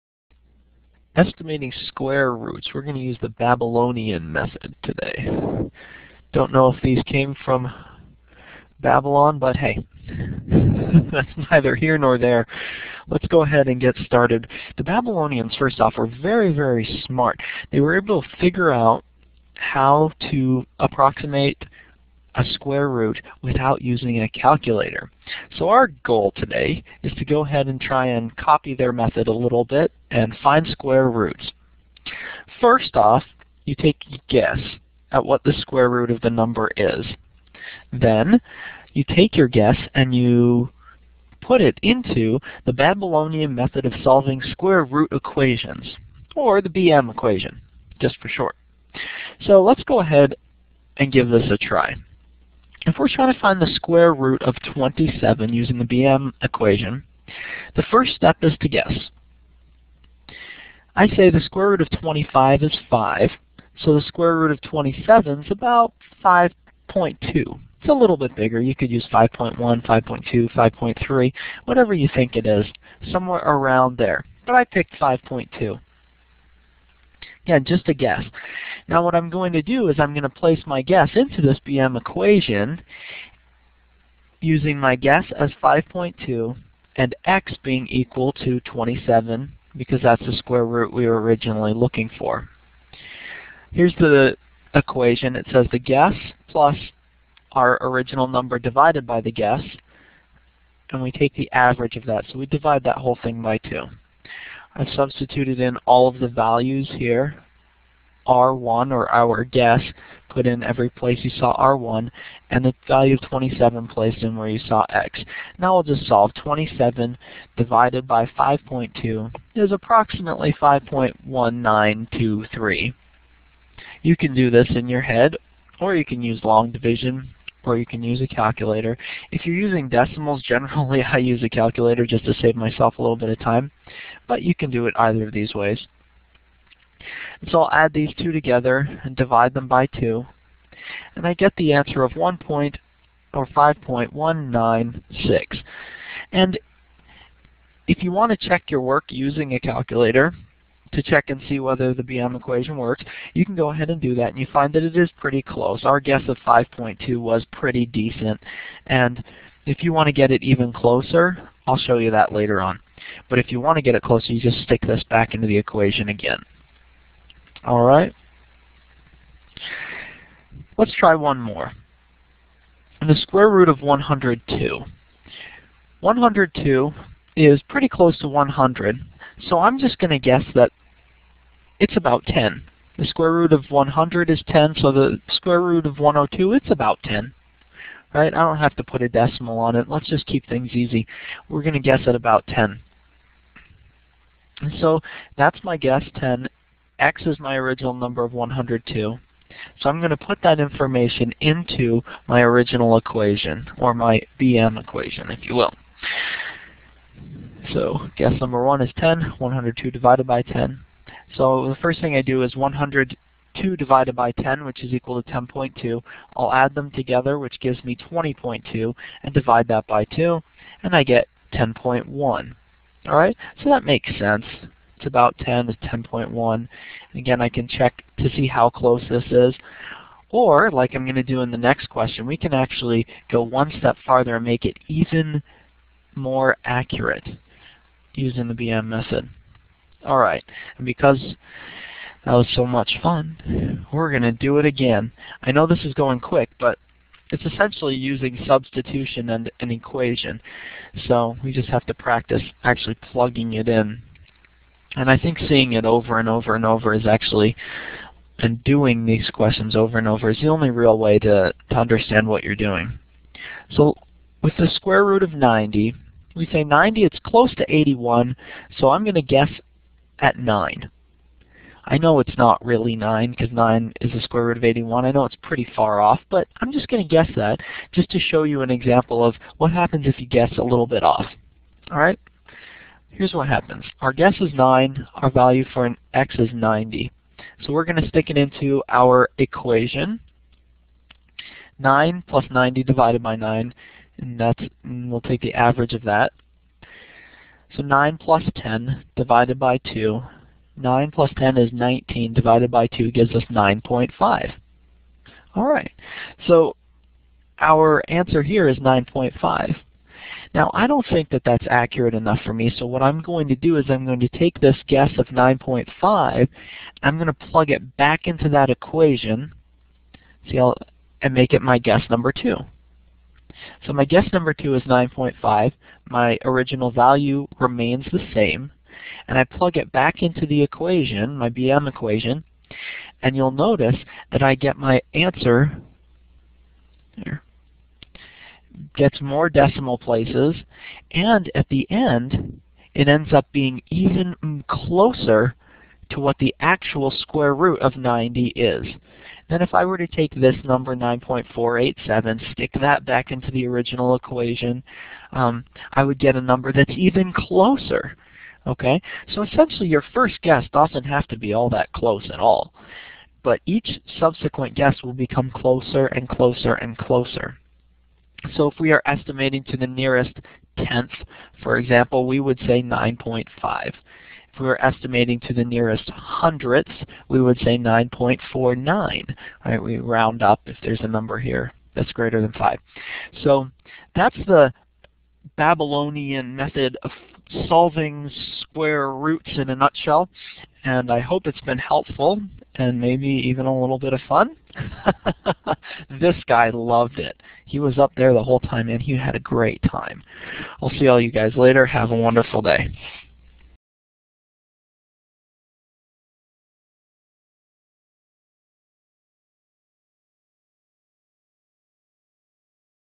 Solving Square Roots: Babylonian Method (Simplifying Math)
Mini-lesson on solving Square roots using the Babylonian method.